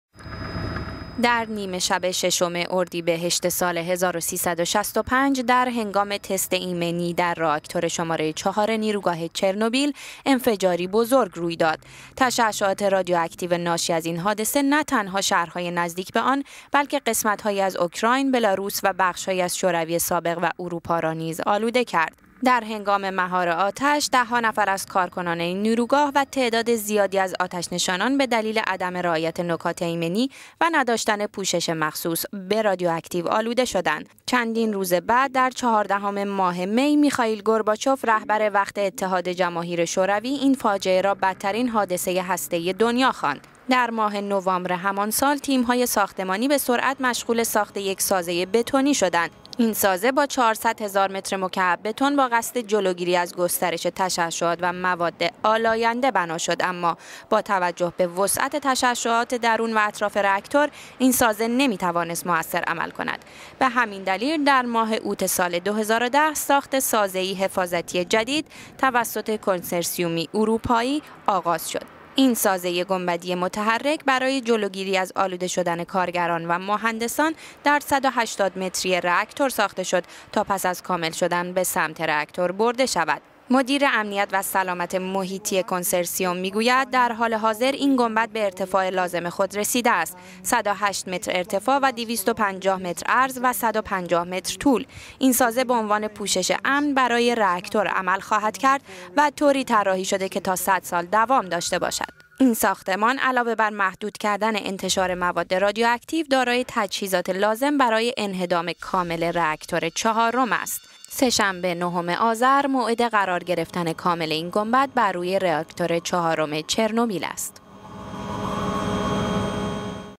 قرار گرفتن گنبد محافظ بر روی رآکتور چهارم نیروگاه چرنوبیل
در نیمه شب ششم اردیبهشت ۱۳۶۵ (۲۶ آوریل ۱۹۸۶) در هنگام تست ایمنی در رآکتور شماره چهار نیروگاه چرنوبیل، انفجاری بزرگ روی داد.

پریپیات، شهری در استان کی یف پایتخت اوکراین و در نزدیکی مرز بلاروس بود. این شهر که با اسکان کارکنان این نیروگاه شکل گرفته بود، با انفجار رآکتور چهارم نیروگاه که در چهارده کیلومتری شمال شهر قرار داشت خالی از سکنه شد. تشعشات رادیواکتیو ناشی از این حادثه نه تنها این شهر بلکه قمست هایی از اوکراین، بلاروس و بخشهایی از شوروی سابق و اروپا را نیز آلوده کرد.